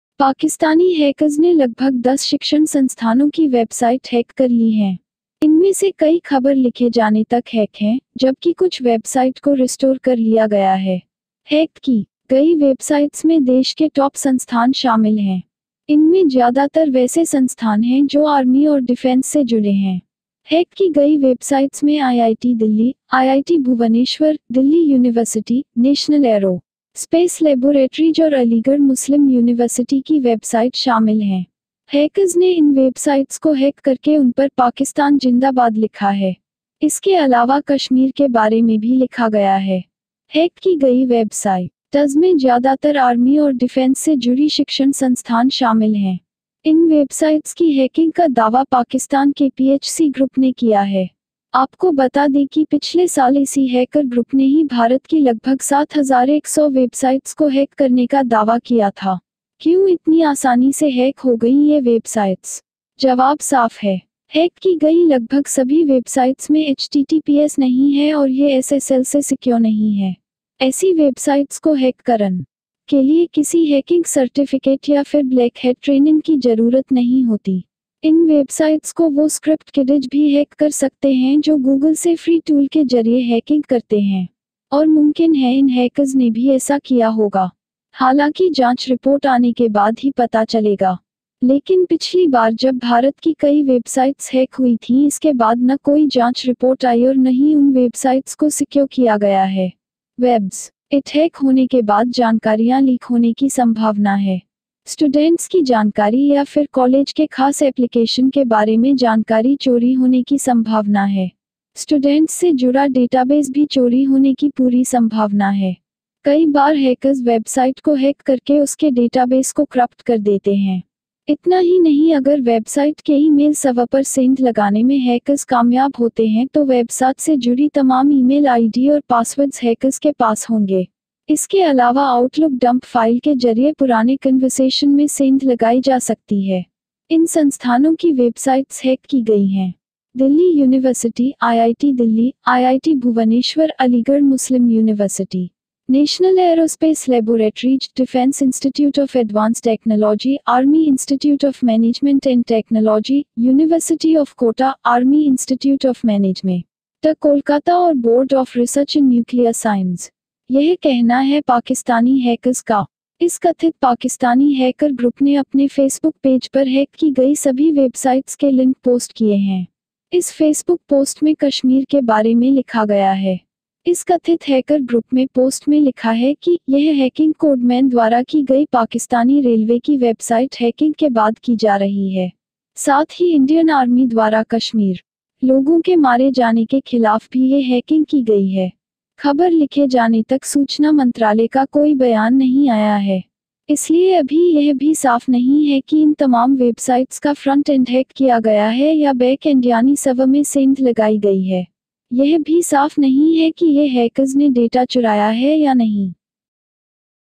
PAKISTANI hackers ने IIT दिल्ली-DU-AMU aligarh muslim uni. समेत कई यूनिवर्सिटी की वेबसाइट्स हैक की
Source aajtak and other news agencies